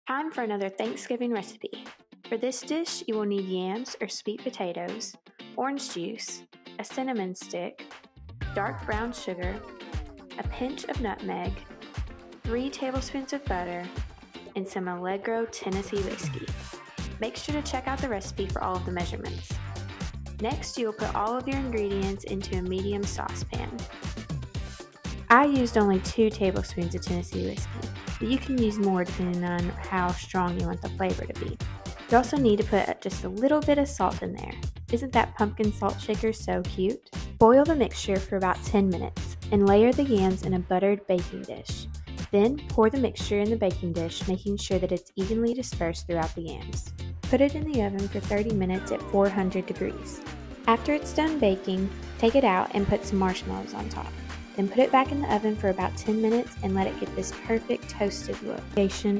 Tennessee Whiskey Yams
This recipe is a neat variation to the traditional yams/sweet potato Thanksgiving dish!